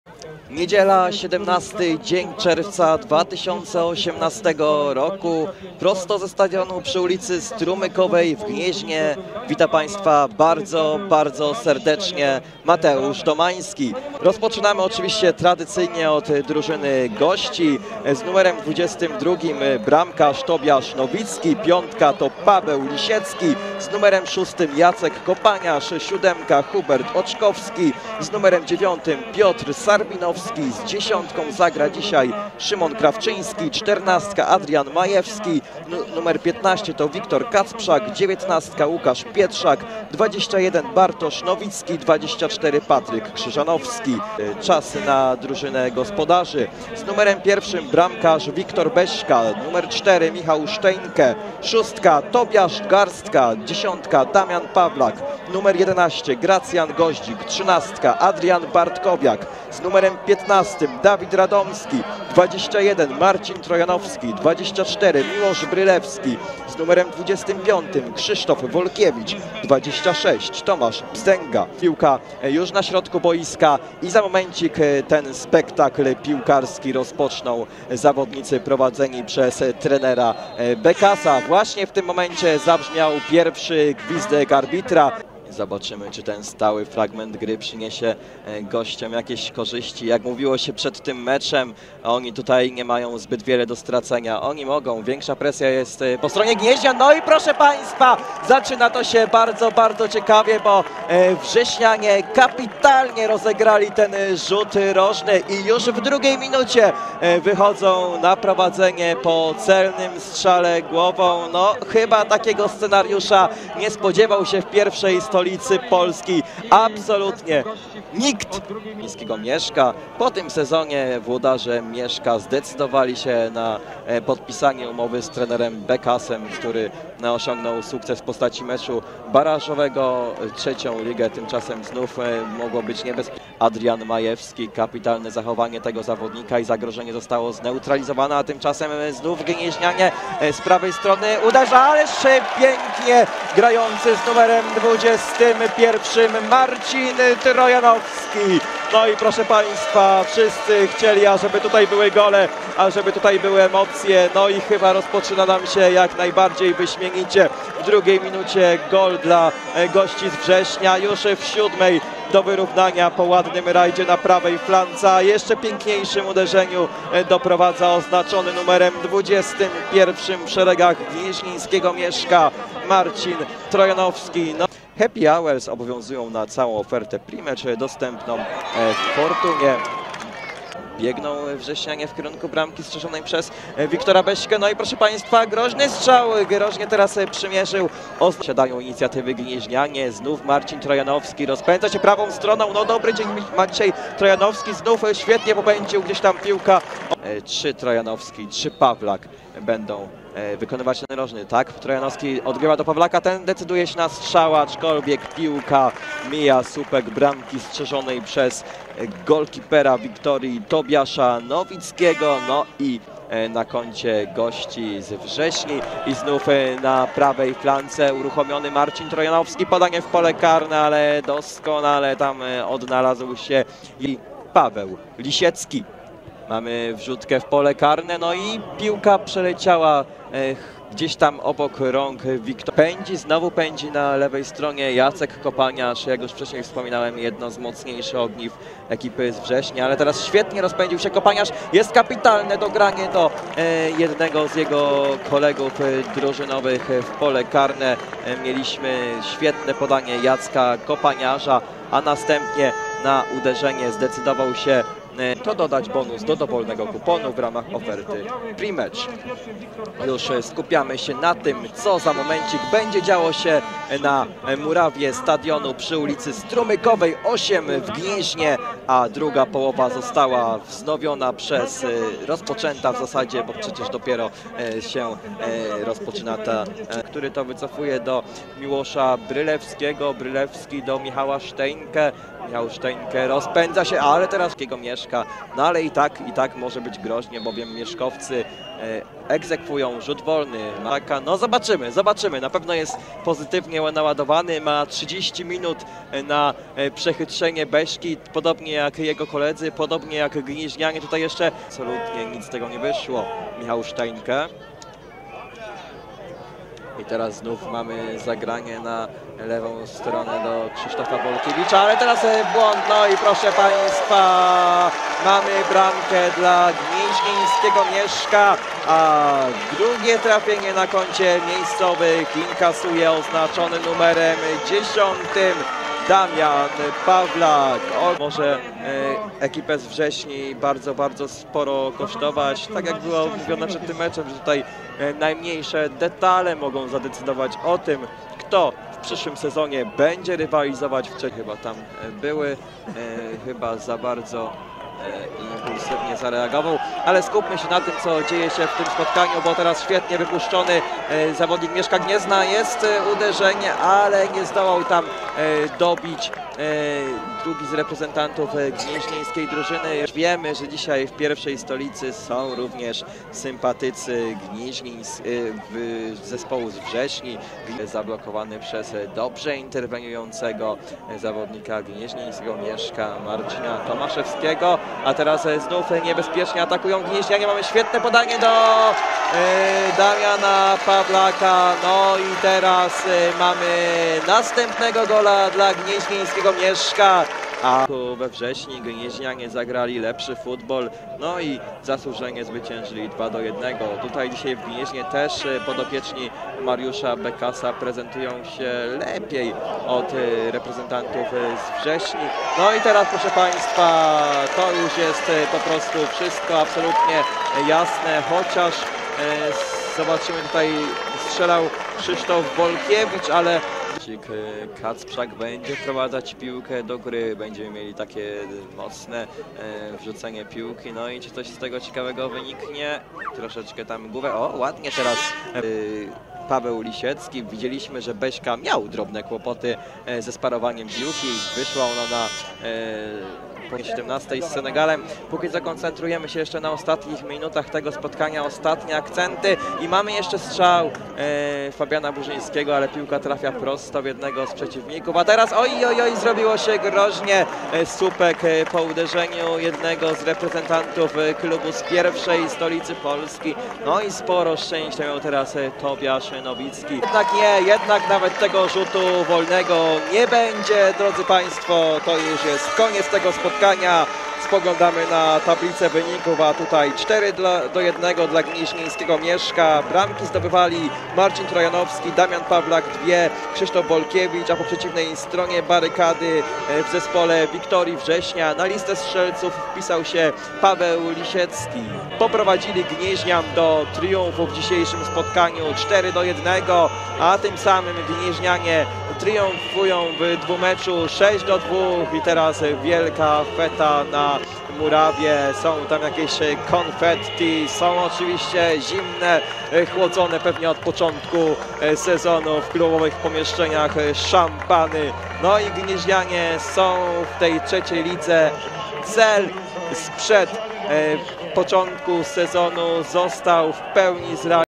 Mieszko Gniezno - Victoria Września 4:1 / SKRÓT / Baraż o III ligę gr. 2 [17.06.2018]
Zobacz najciekawsze akcje z rewanżowego meczu barażowego o awans do 3 ligi pomiędzy Mieszko Gniezno - Victoria Września z KP Sport »